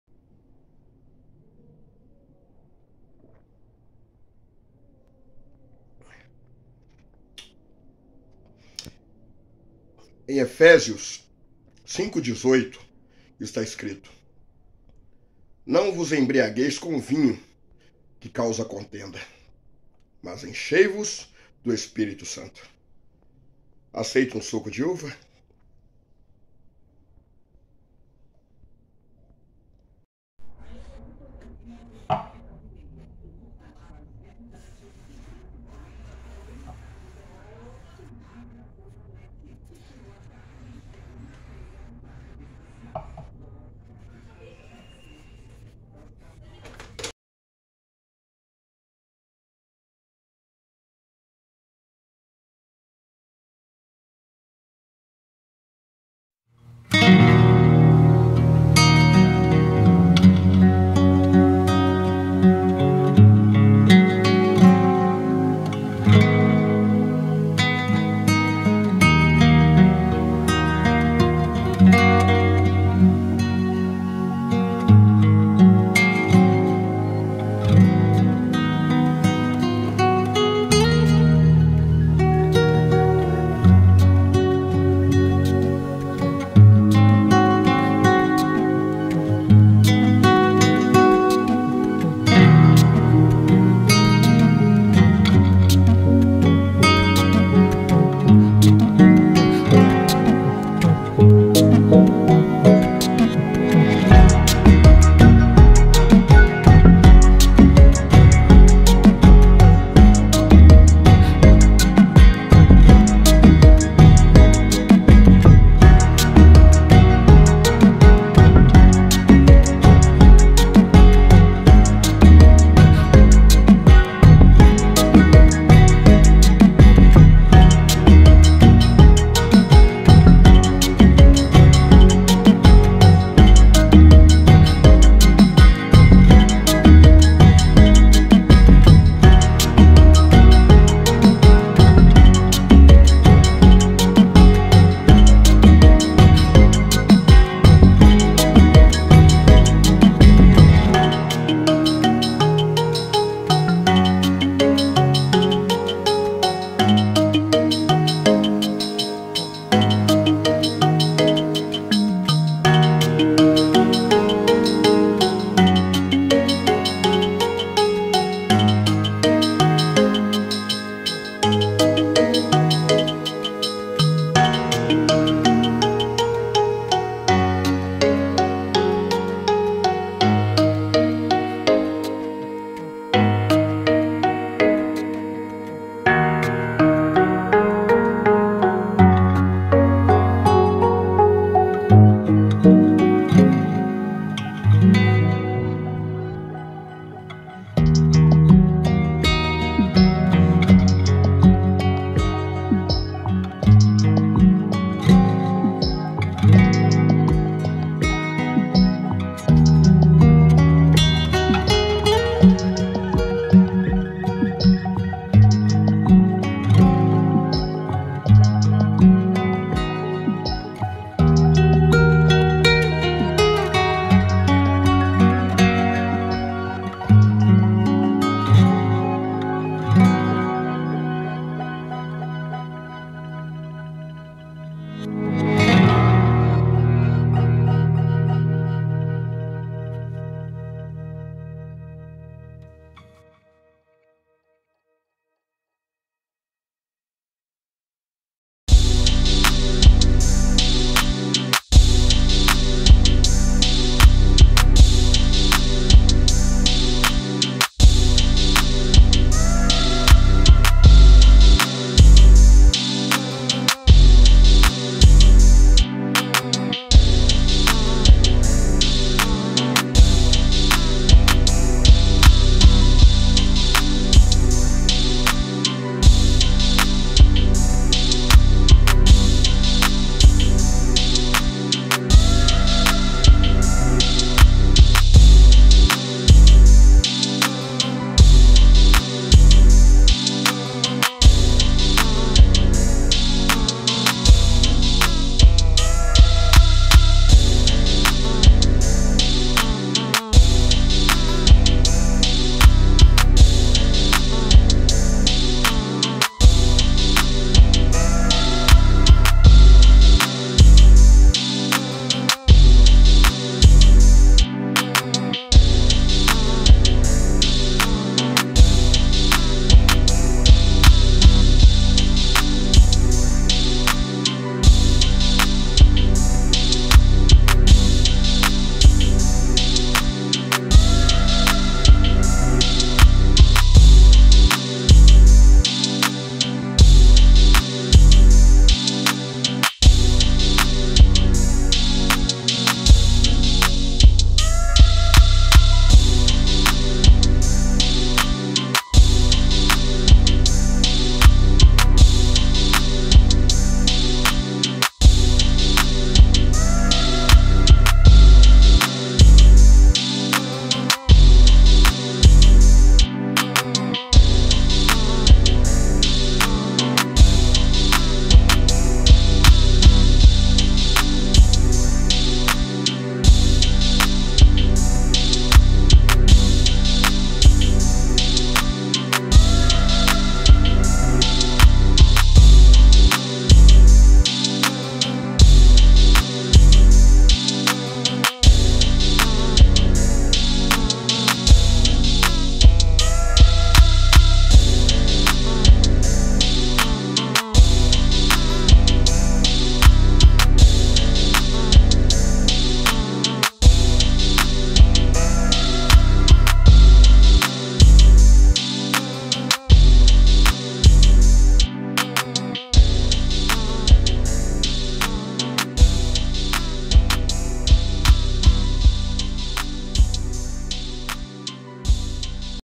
o que fazer com uma taça de Vinho
Terrário na taça de vinho
Mini jardim.